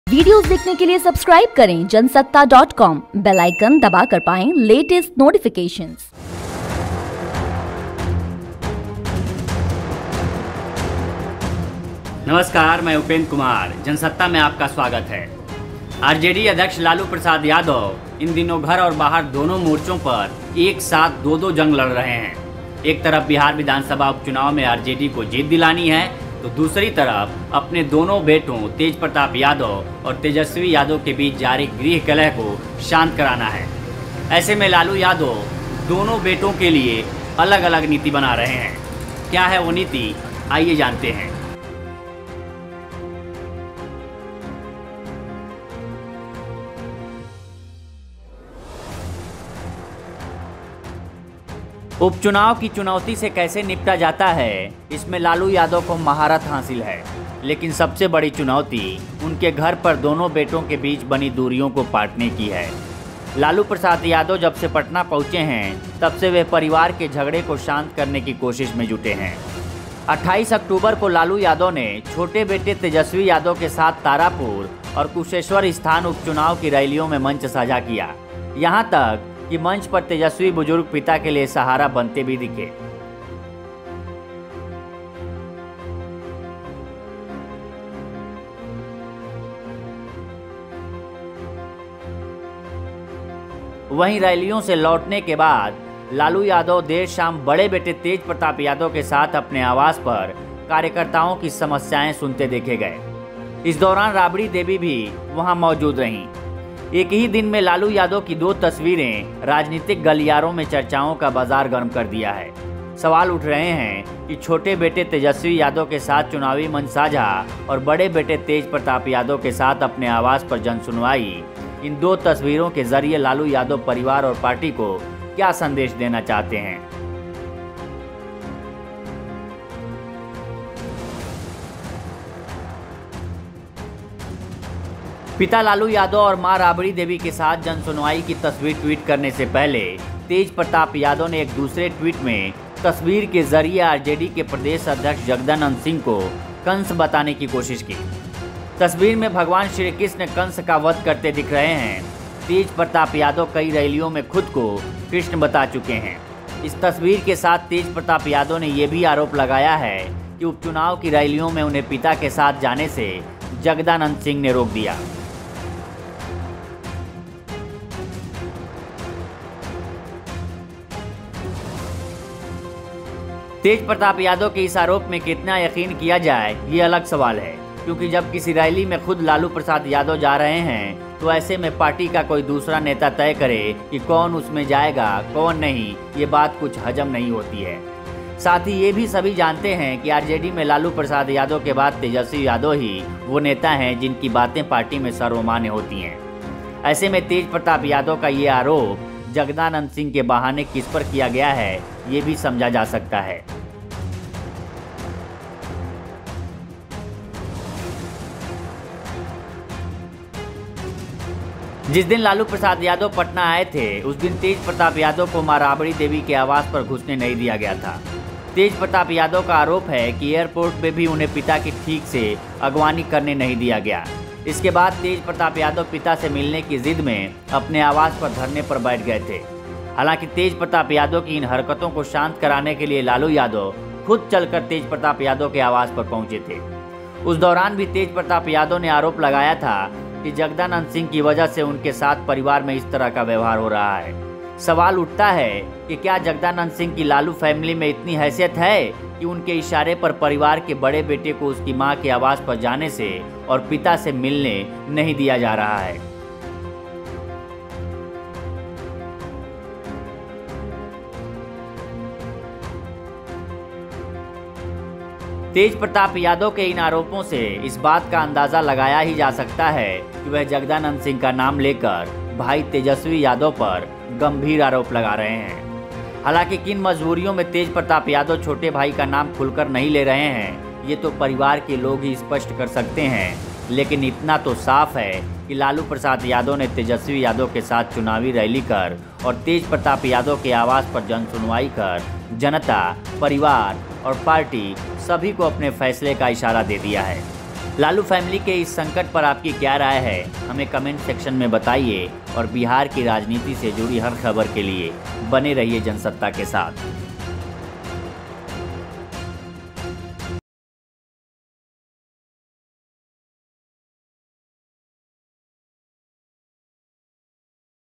Tejashwi संग चुनाव प्रचार और Tej Pratap संग जनसुनवाई, Lalu Yadav Family में ये क्या हो रहा है?
Tej Pratap-Tejashwi and Family Politics of Lalu Yadav: आरजेडी (RJD) के कुनबे में मचे घमासान को खत्म करने की जिम्मेदारी अब लालू प्रसाद यादव (Lalu Prasad Yadav) ने अपने कंधों पर ले ली है। अपने दोनों बेटों तेज प्रताप यादव (Tej Pratap Yadav) और तेजस्वी यादव (Tejashwi Yadav) के बीच के झगड़े को सुलझाने बिहार पहुंचे लालू यादव (Lalu Yadav) ने दो तस्वीरों के जरिए अपनी रणनीति जाहिर कर दी। पहली तस्वीर में वो तेजस्वी के साथ उप चुनाव (Bihar Election Bypoll) के लिए जनसभा कर रहे हैं तो दूसरी तस्वीर में तेज प्रताप और राबड़ी देवी (Rabri Devi) के साथ जन सुनवाई। जनसत्ता की इस खास रिपोर्ट में एक नजर राजद के इसी अंदरूनी घटनाक्रम पर...